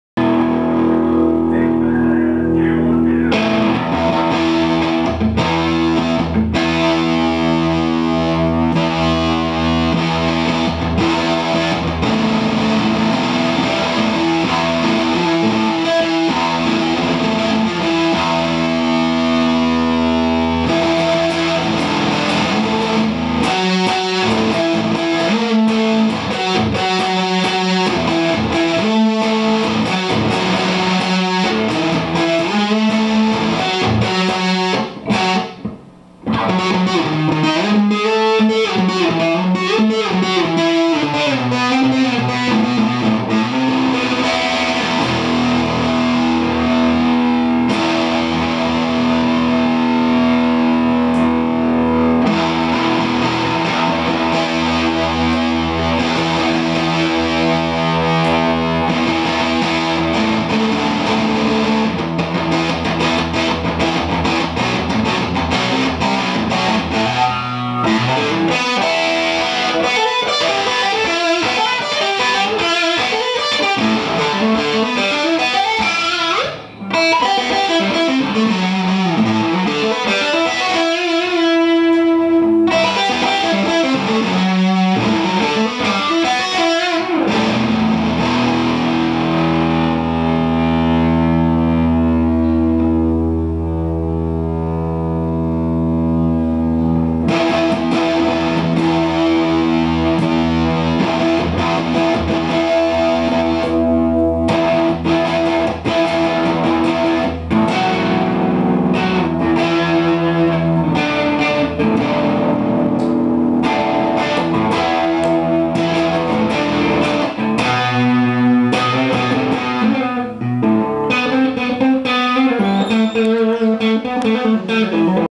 Testing out the amps
On our visit to San Francisco, we were walking through the city and happened upon one of the nicer Guitar Centers you'll find. This is Caleb in the "loud amp room" testing out a Mesa I think. Early he plays a riff from a Minneapolis band he likes, Blue Sky Blackout